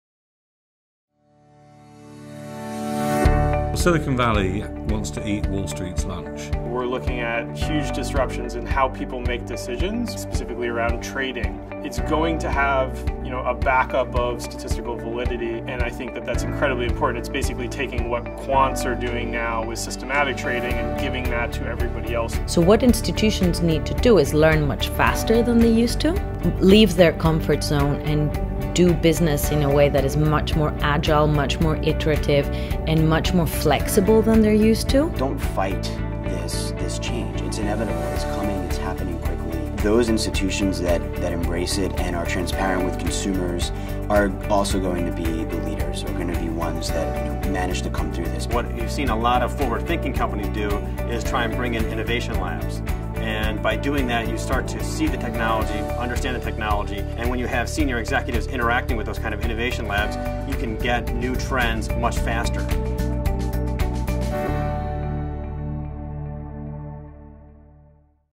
Finance's digital disruption
Experts discuss how digital disruption will affect finance the next five years